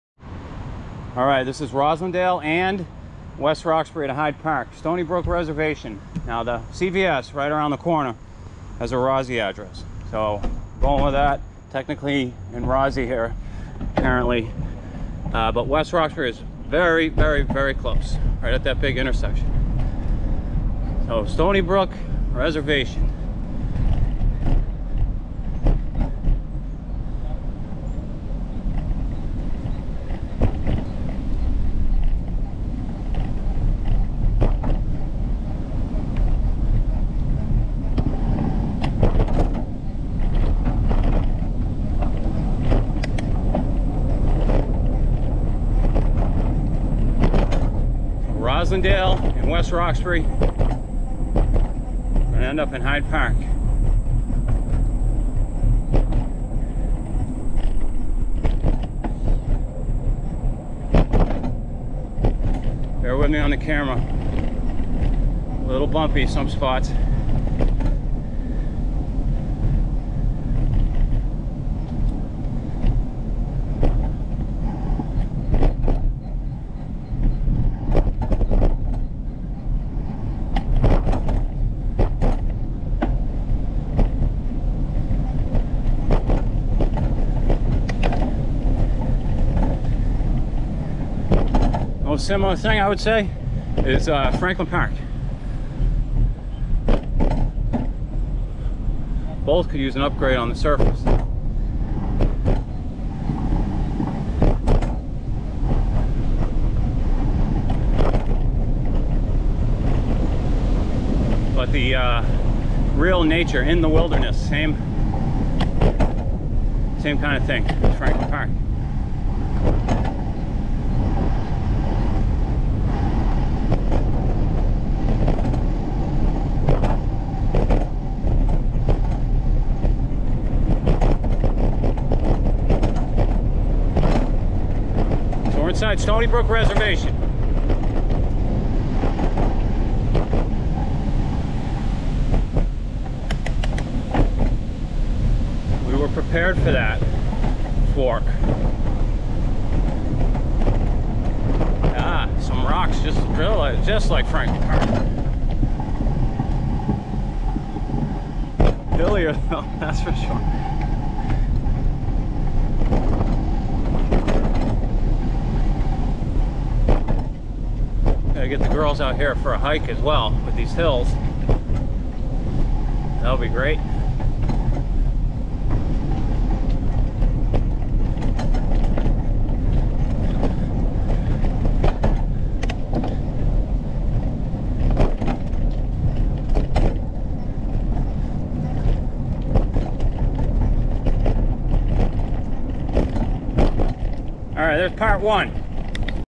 Boston Bike Route - Roslindale/West Roxbury to Hyde Park pt 1
stony brook path that cvs close by has roslindale addy make sure you check and look on how to get to this entrance and inside the stony brook reservation overall . . . . . . . wild ride inside here check it out

came straight down washington to arrive the wind was brutal and metropolitan hill was not much fun, tough wind this entire day rozzie, westie, hyde park